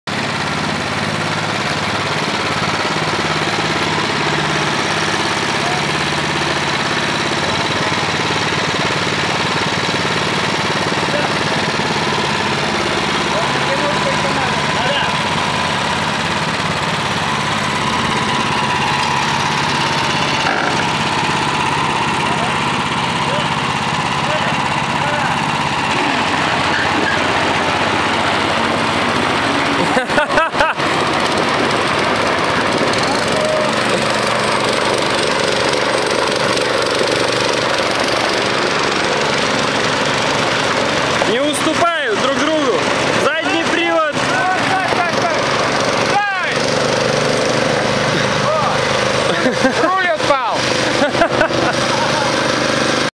Соревнования мнитракторов. Мастерская КТЗ проводит конкурс самодельных минитракторов.
Самоделки своими руками. Соревнуются самодельные тракторов.
Оба трактора собраны своими руками
У 1-го привод 4*4 переломка, без блокировки
У 2-го привод задний, заварен дифер.
В процессе перетягивания каната ни один из мини тракторов не уступал другому. Хотя один трактор полноприводный, а другой заднеприводный.
Зато в самом разгаре конкурса у одного из тракторов отпар руль))
Смотрите до конца не пожалеете!

Следите за каналом! Благодарю за просмотр!